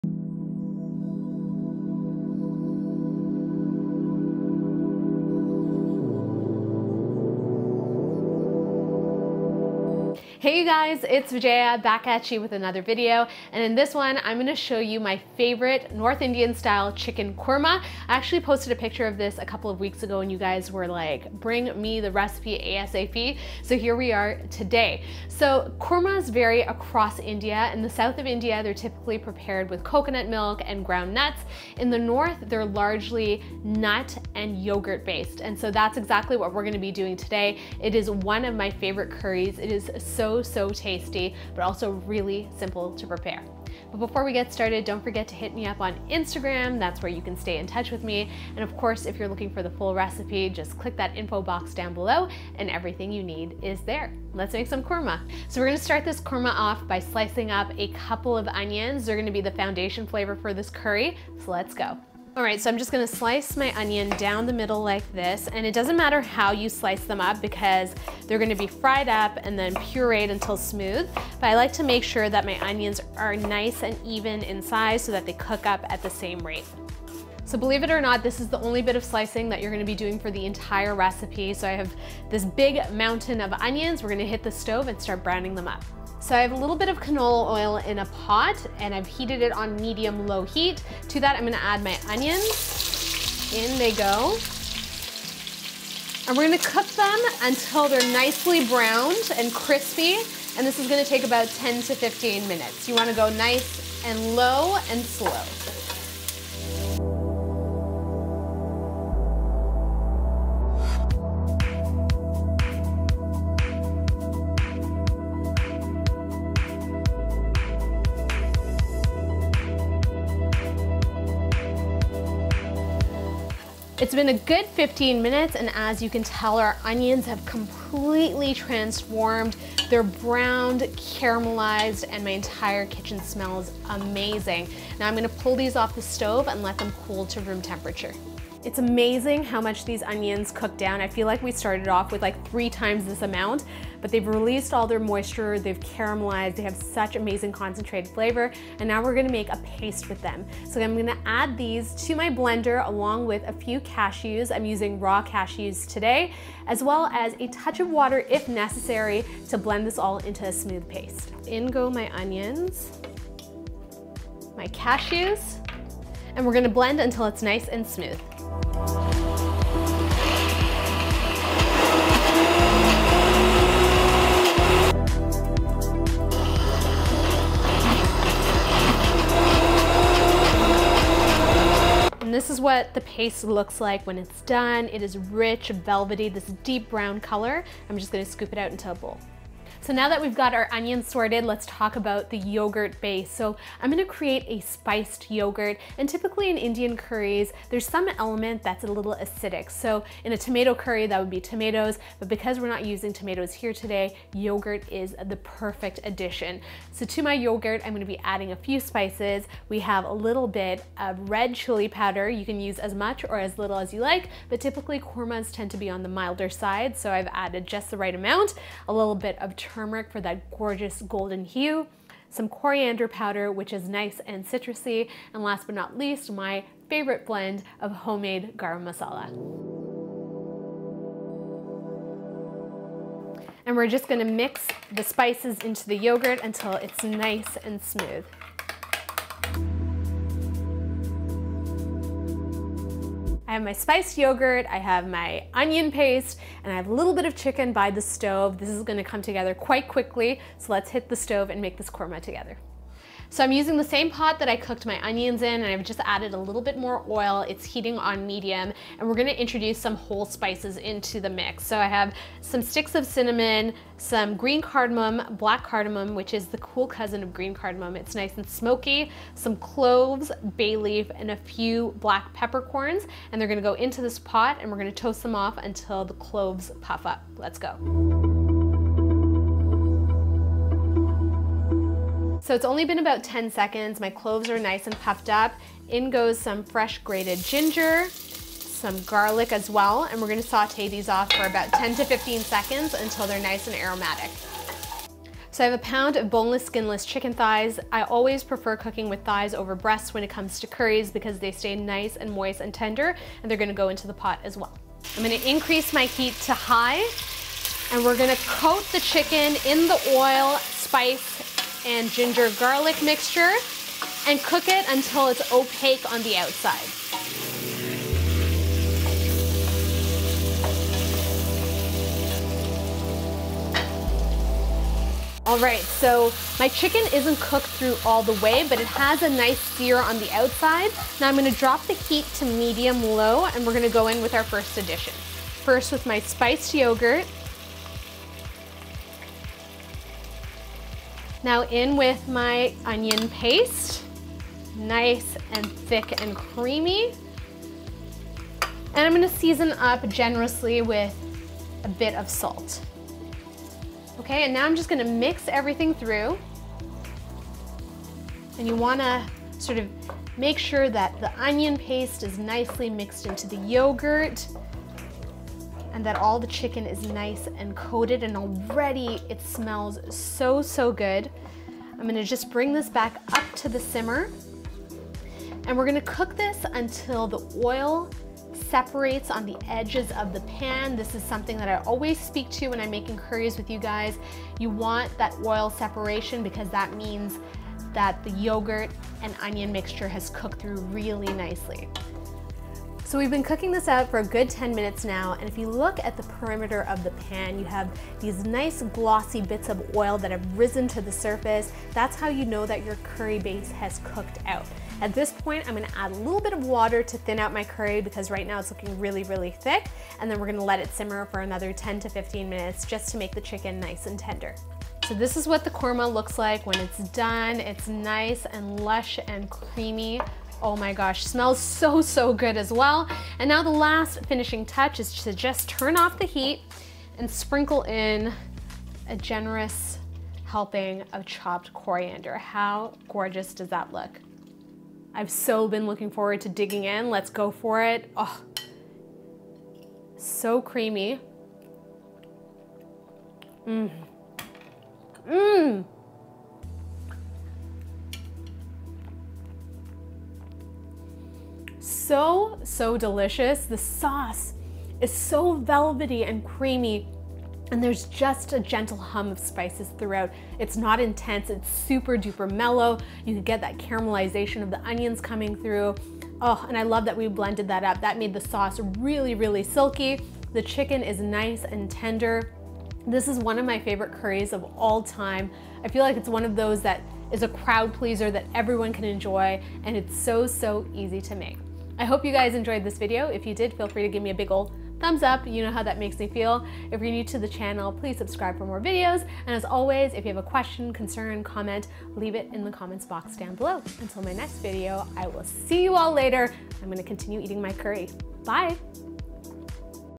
North Indian Chicken Korma
This Chicken Korma is creamy, and velvety with the perfect hum of spices, and is definitely up there as one of my favourite curries of all time!

The caramelized onions and spiced yogurt give this korma such an amazing depth of flavour. Great over hot basmati rice or served with warm chapathis. Enjoy! :)

Full recipe listed below. I hope you enjoy this video :)

Let's be friends :D

RECIPE

INGREDIENTS

5 tbsp, Canola Oil
2 medium Onions, chopped
10 raw Cashews
1 cup, Whole Yogurt
1/2 tsp, Turmeric
1 tsp, Coriander Powder
1 tsp, Red Chilli Powder
2 tsp, Garam Masala
1 stick of Cinnamon
3 Green Cardamom Pods
1 Black Cardamom
5 Cloves
10 Black Peppercorns
1 Bay Leaf
1 tsp, finely grated Ginger
1 tsp, finely grated Garlic
1 lb, boneless skinless Chicken Thighs cut into big chunks
1 cup, Water
Salt to taste
Fresh chopped Coriander to garnish

METHOD

Place 2 tbsp of oil in a heavy bottom pot, and heat on medium-low. Add onions and slowly sauté for 10-15 minutes until deeply browned. Allow to cool, and transfer to a blender with cashews. Blend until smooth, adding a little bit of water if necessary to form a thick paste.

In a bowl, whisk together yogurt, turmeric, coriander powder, red chilli powder, and garam masala until fully mixed. Set aside.

Heat remaining 3 tbsp in same pot on medium-low heat. Add cinnamon, green and black cardamom pods, cloves, black peppercorns, and bay leaf. Sauté for 15-30 seconds or until cloves have puffed up. Then add grated ginger and garlic and fry for an additional 30 seconds.

Increase heat to high, and add chicken. Cook for 5-7 minutes, stirring frequently until the chicken no longer looks raw on the outside.

Stir in spiced yogurt, onion paste, and salt to taste and reduce heat to medium-low. Partially cover with a lid and cook for 10-12 minutes, stirring often until the oils begin to separate along the edge of the curry.

Add water, and increase heat to medium. Cook for an additional 15-20 minutes partially covered until the chicken is tender and cooked through.

Remove from heat and garnish with fresh chopped coriander leaves. Enjoy! :)

MUSIC